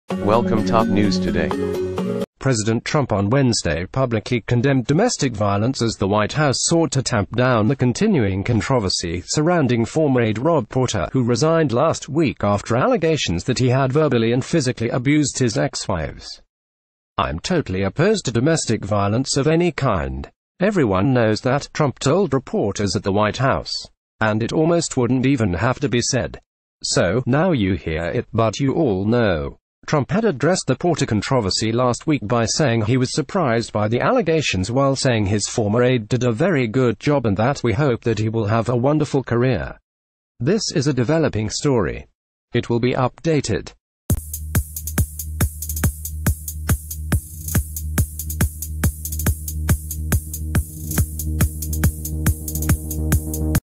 BREAKING: Trump says he is ‘totally opposed to domestic violence of any kind’ days after aide is ac
BREAKING: Trump says he is ‘totally opposed to domestic violence of any kind’ days after aide is accused of abusing his ex-wives
President Trump on Wednesday publicly condemned domestic violence as the White House sought to tamp down the continuing controversy surrounding former aide Rob Porter, who resigned last week after all...